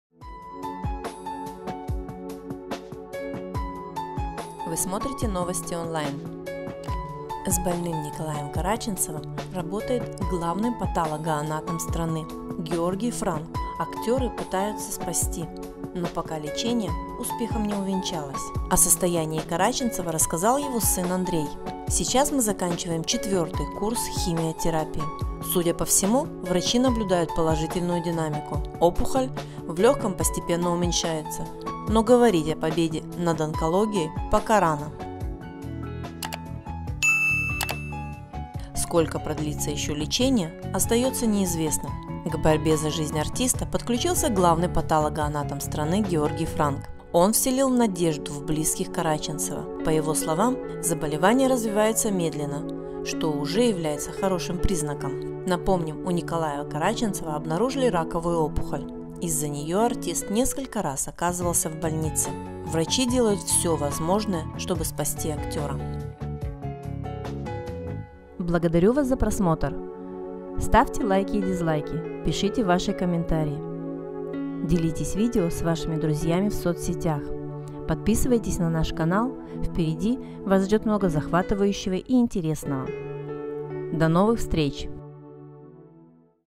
К Караченцову позвали патологоанатома! (23.01.2018)
К Караченцову позвали патологоанатома!
С больным Николаем Караченцовым работает главный патологоанатом страны Георгий Франк. Актера пытаются спасти, но пока лечение успехом не увенчалось.